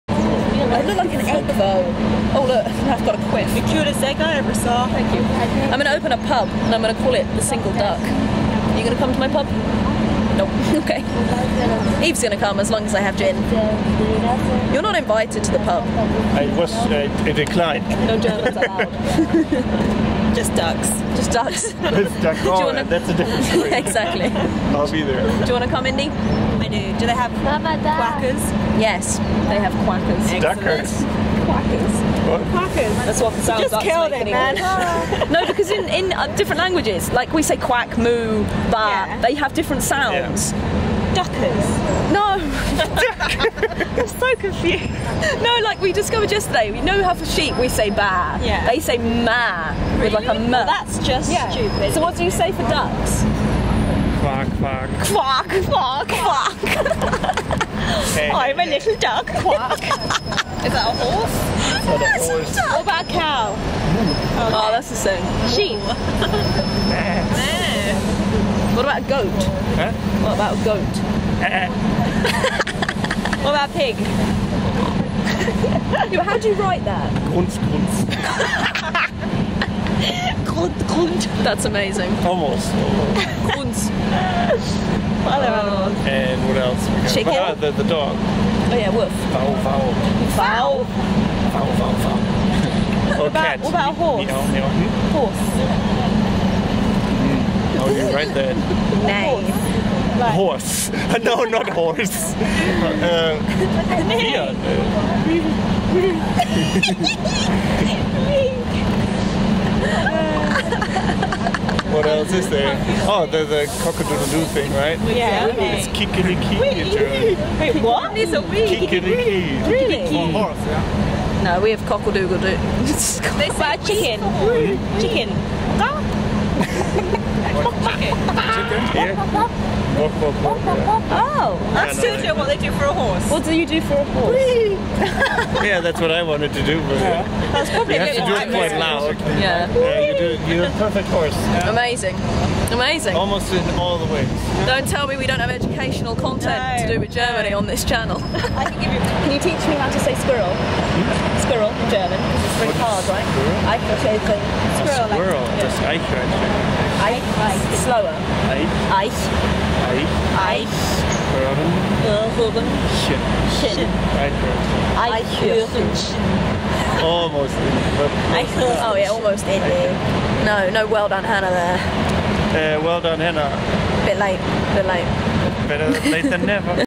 Germans Make Weird Sounds...
GUTEN TAG
Today we have some fun content regarding the different noises that animals make... IN GERMAN!
We realised that the very noises animals make e,g. oink, woof, neigh in English, is different in German. Which is something I'd never really thought about!
Here are a few examples of the differences!
Sorry the sound quality isn't great. We were on a bus in the middle of London, so it is rather noisy!

A short video, yes. BUT it is a midweek one that I don't usually upload ;)  I'm currently in Berlin so look out for a Vlog coming this Friday!